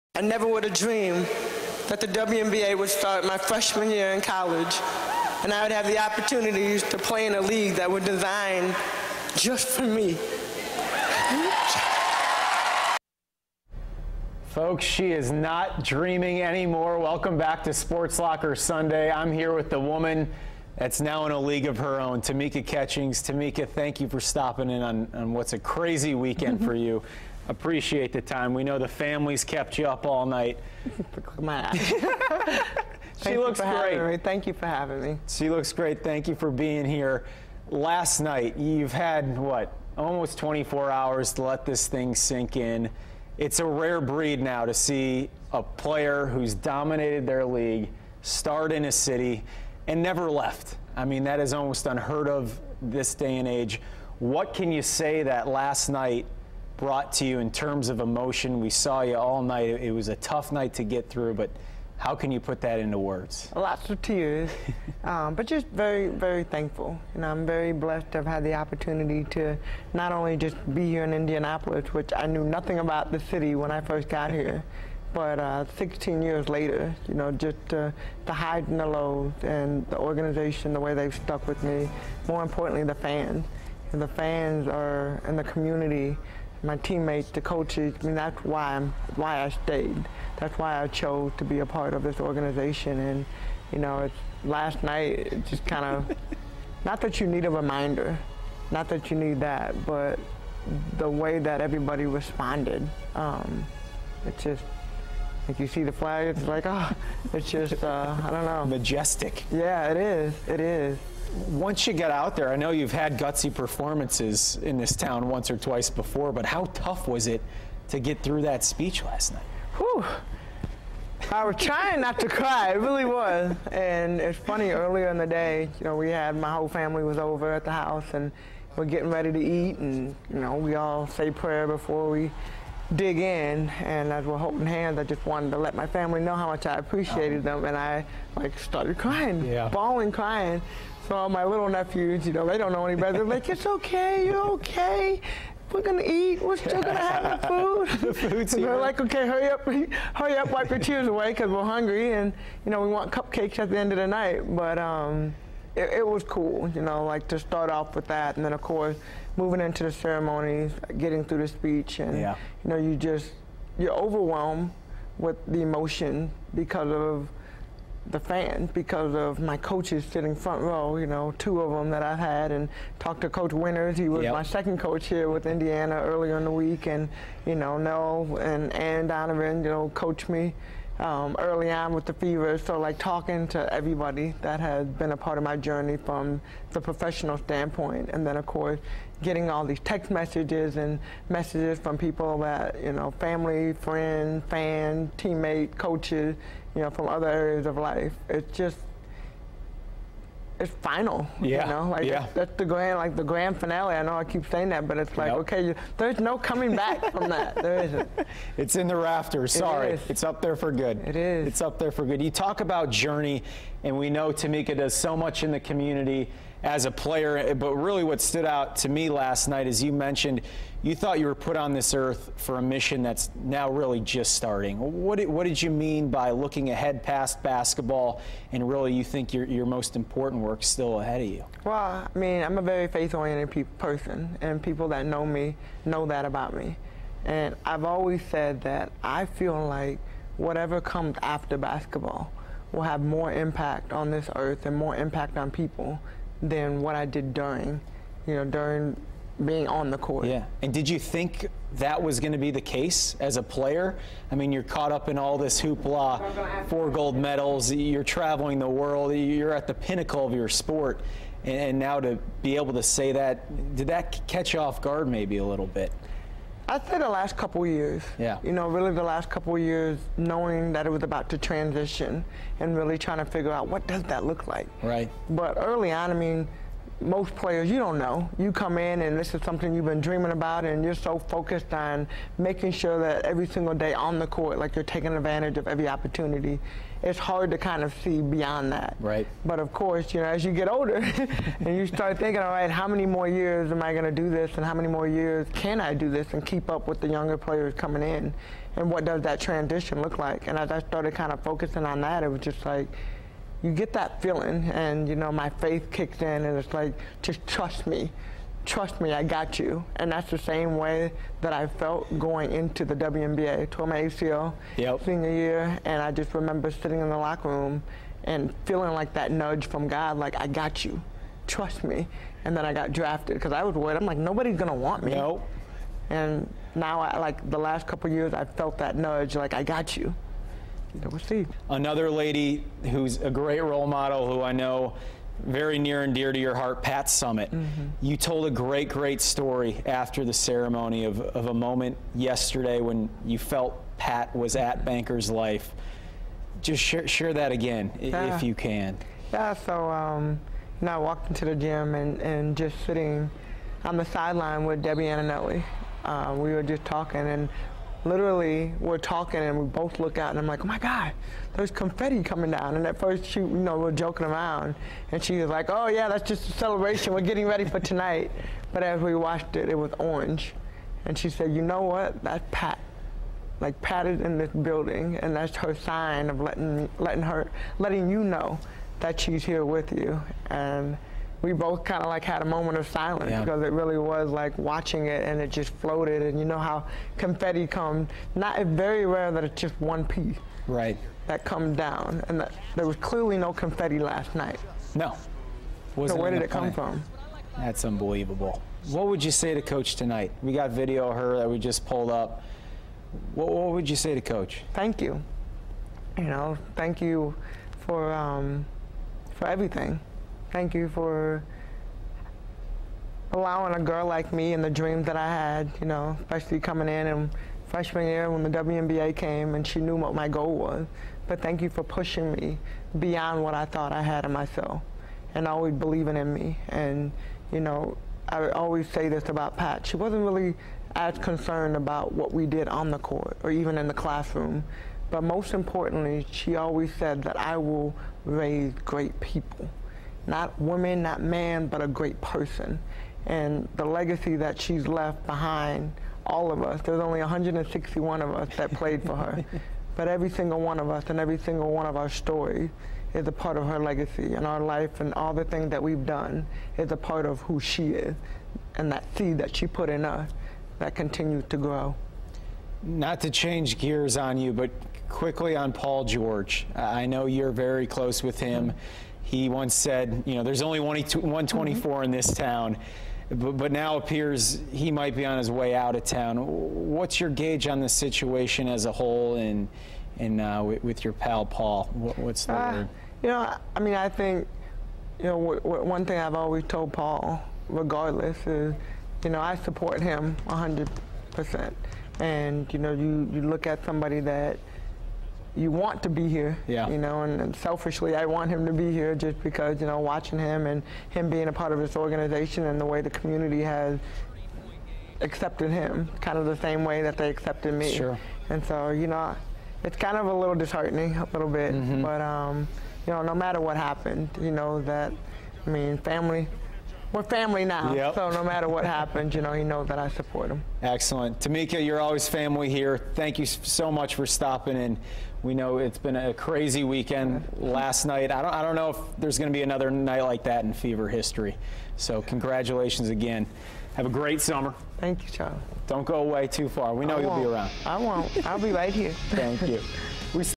Tamika Catchings talks career, legacy and Paul George plans
WISH-TV's SportsLocker Sunday